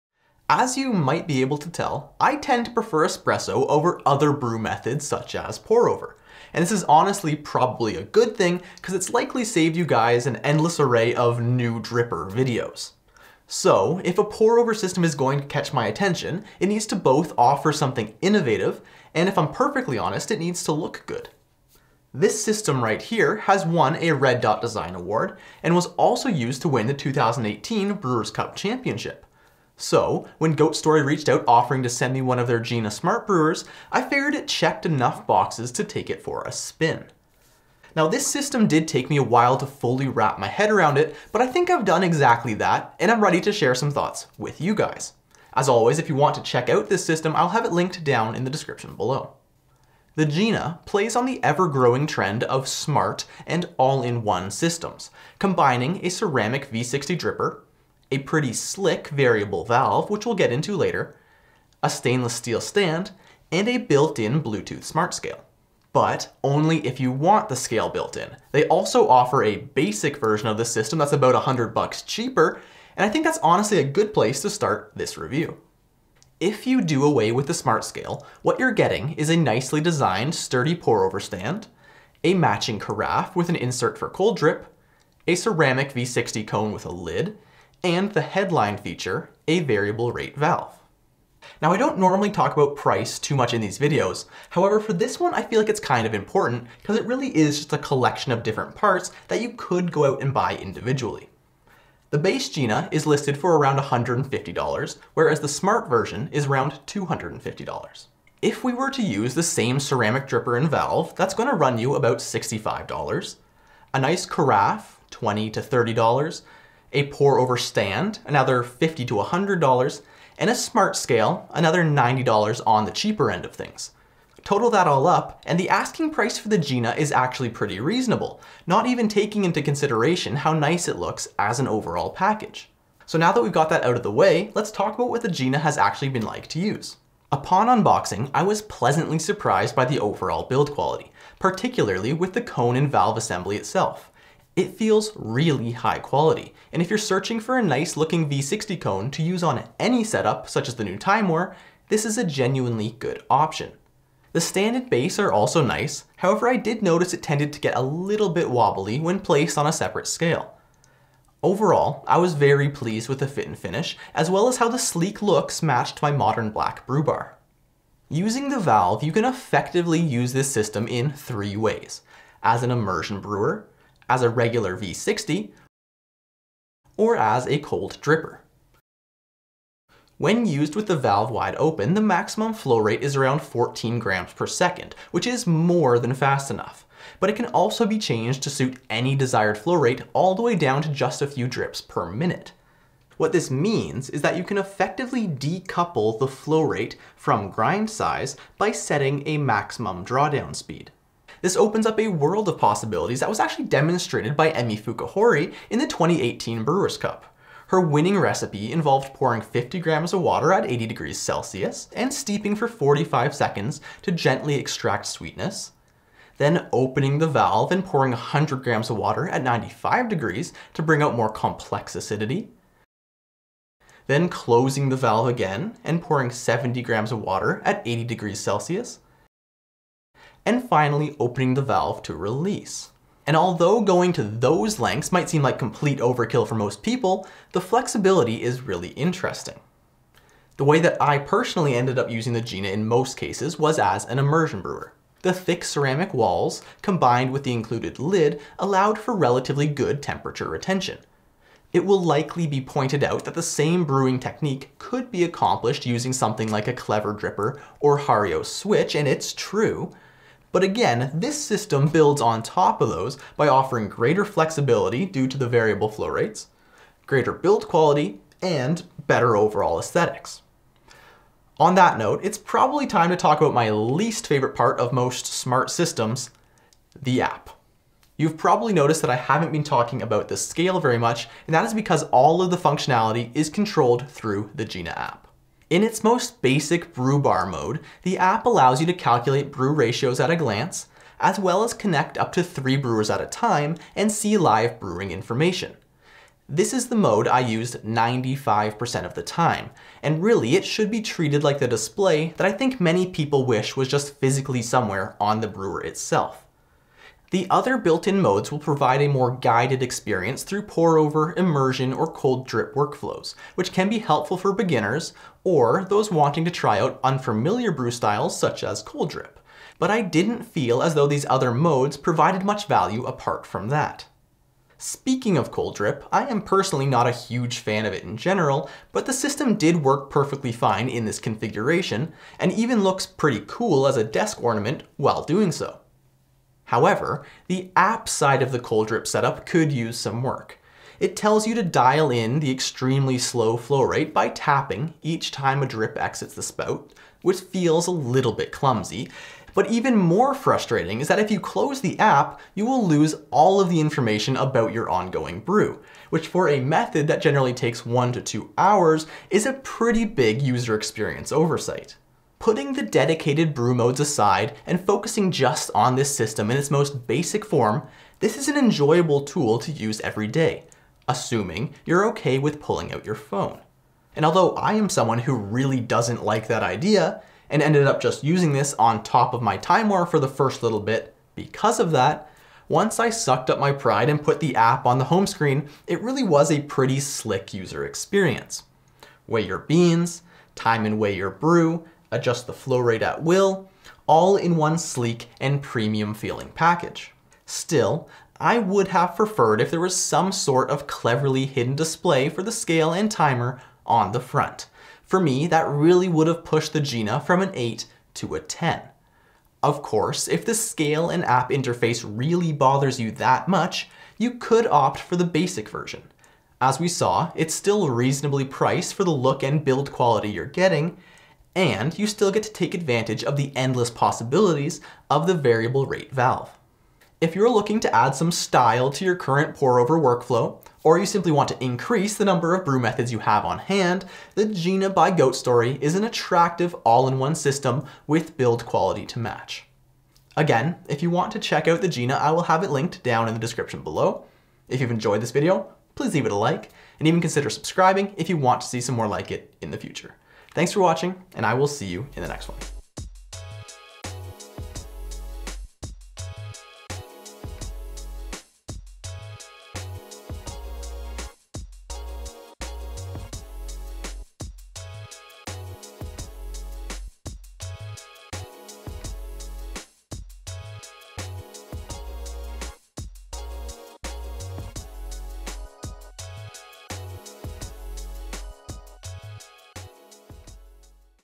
Goat Story GINA | Smart Brewer Review
GINA Smart Brewer

Goat story has risen quickly within the specialty coffee world offering well designed, and sometimes quirky, coffee related products. The GINA smart brewer promises build quality, brew quality, and convenience. In todays review, we put GINA to the test, and see if this pour over brewer really is a "smart brewer".

0:00 - Intro
0:50 - Overview
1:31 - Price Breakdown
2:13 - Build Quality / Design
2:48 - Brewing Experience
4:30 - The App
6:02 - Conclusion